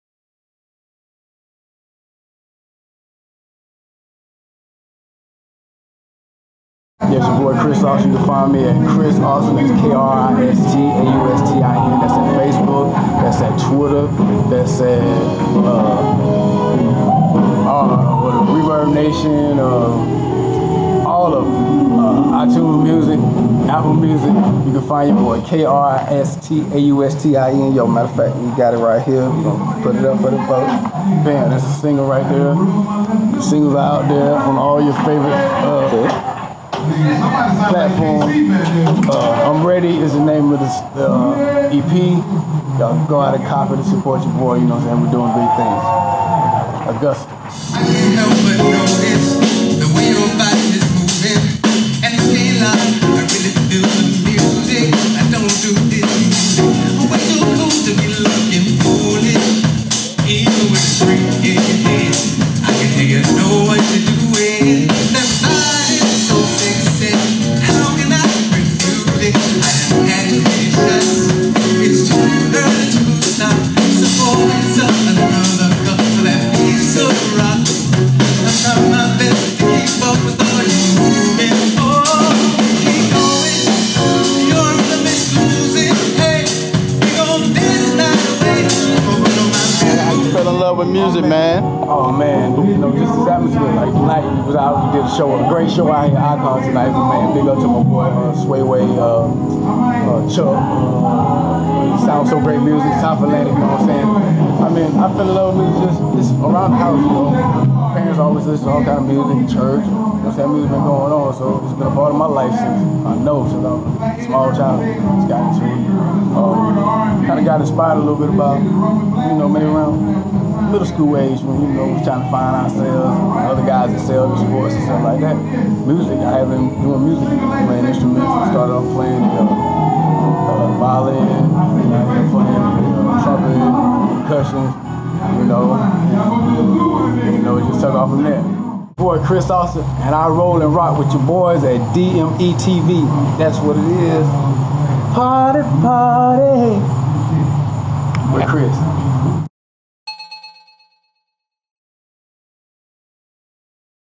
Nothing But Grateness Concert ft. KRIST AUSTIN | LIVE PERFORMANCE
Checkout @kristaustinent performance clip and interview with KevDaCeo of DMEtv from nothingbutgrateness concert.